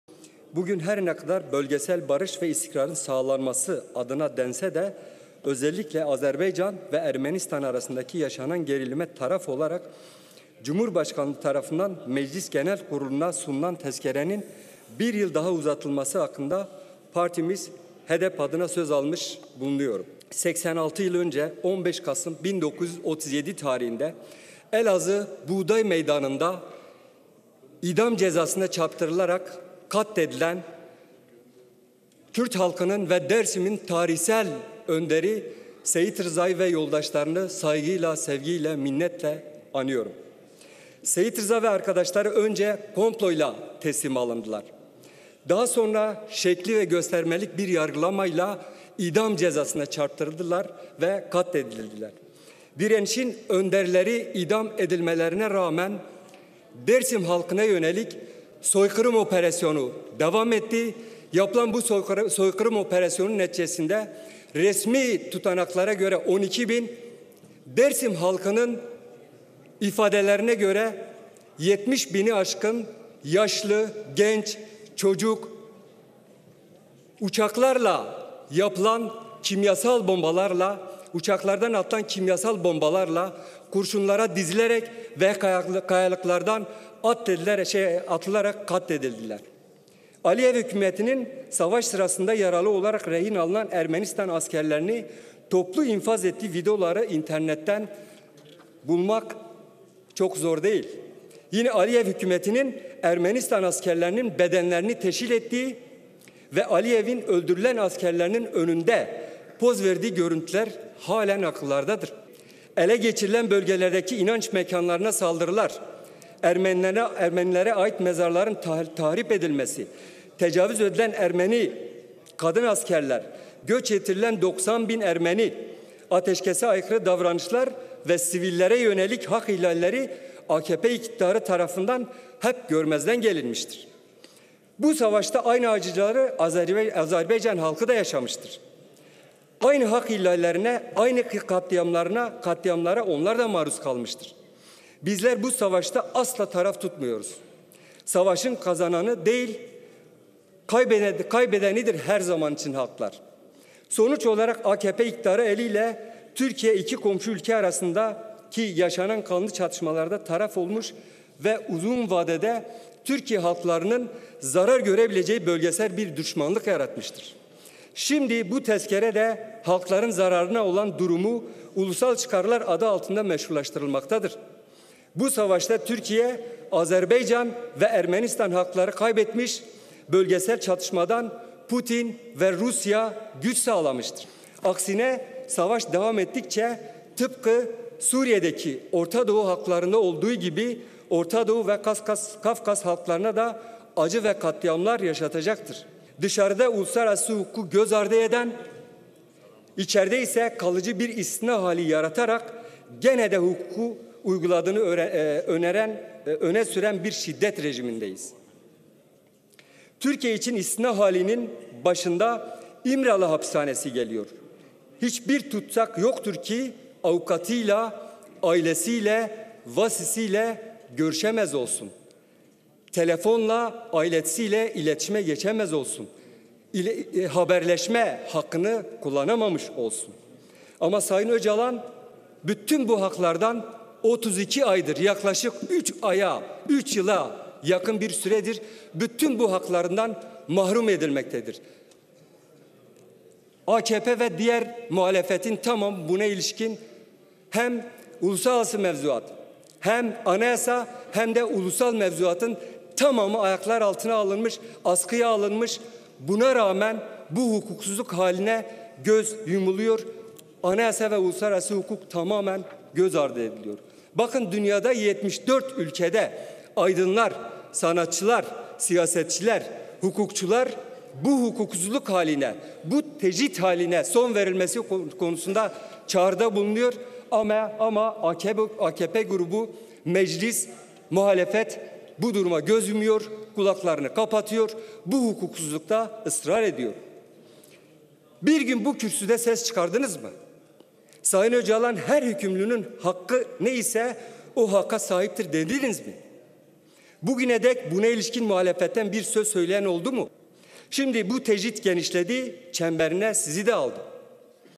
ABDULLAH ÖCALAN’IN ‘UMUT HAKKINA’ VURGU YAPTI
AİHM’in PKK Lideri Abdullah Öcalan’a ilişkin verdiği ihlal kararına değinen HEDEP’li Serhat Eren, “Cezanın gayri insaniliğini ne zaman konuşmaya başlayacaksınız?” diye sorarak, tepki gösterdi.

Halkların Eşitlik ve Demokrasi Partisi (HEDEP) Amed Milletvekili Serhat Eren, Azerbaycan’a asker göndermeye ilişkin tezkere görüşmelerinde, 25 yıldır İmralı Adası’nda bulunan F Tipi Yüksek Güvenlikli Kapalı Cezaevi’nde ağır tecrit altında tutulan ve 32 aydır kendisinden haber alınması engellenen PKK Lideri Abdullah Öcalan durumuna dikkat çekti.